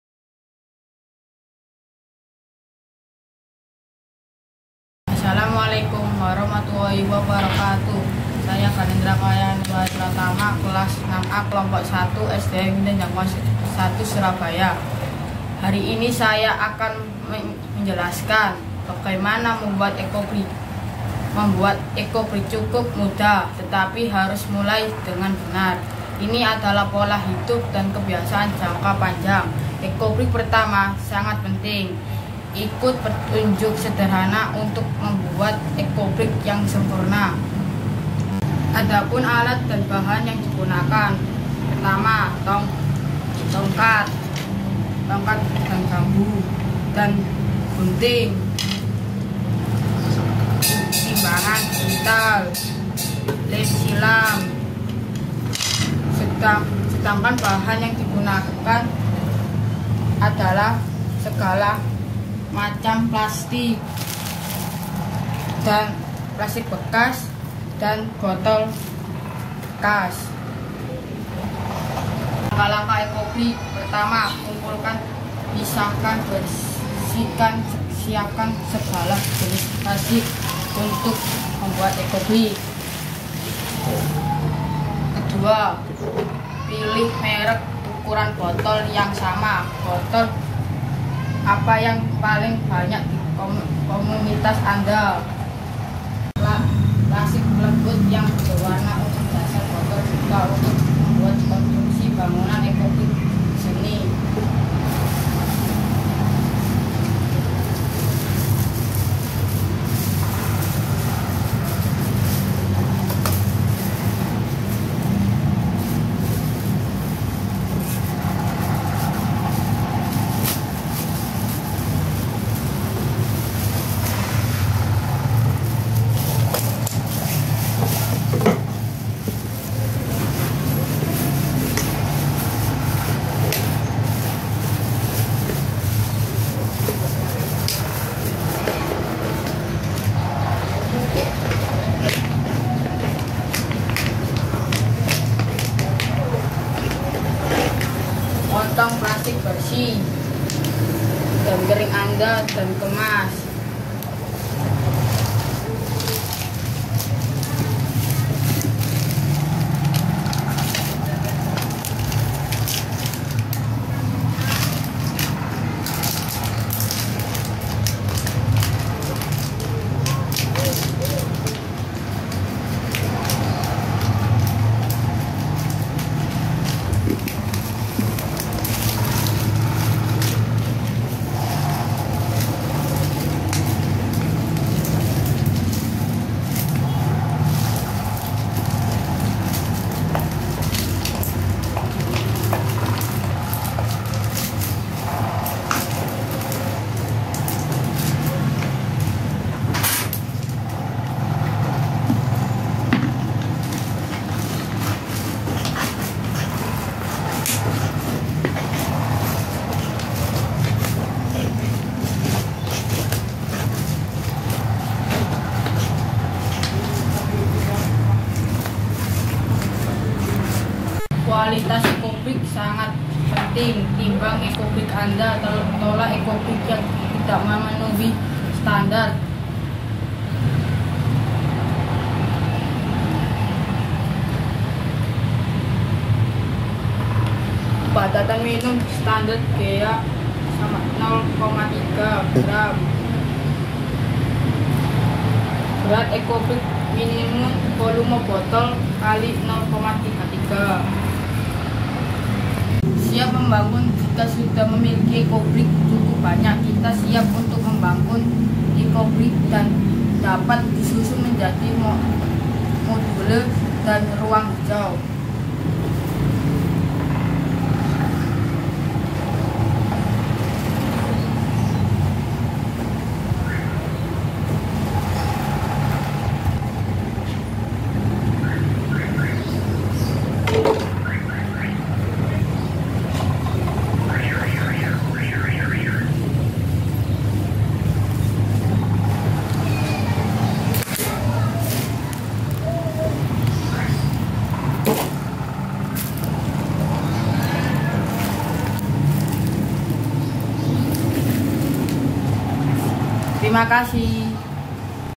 GANENDRA 2023, ECOBRICK SEBAGAI SOLUSI PENANGGULANGAN MASALAH LIMBAH PLASTIK, NGINJAS SURABAYA
UNJUK KERJA DAN KARYA (UKK) KELAS 6

Melestarikan lingkungan alam (bersama)
ECOBRICK SEBAGAI SOLUSI PENANGGULANGAN MASALAH LIMBAH PLASTIK
Kelompok 1
 Ganendra Kaia Nurwahid Pratama

Guru Pembimbing :
Ibu Naini Dwi Rachmawati, S.Pd.

SDN NGINDEN JANGKUNGAN I / 247 SURABAYA
2023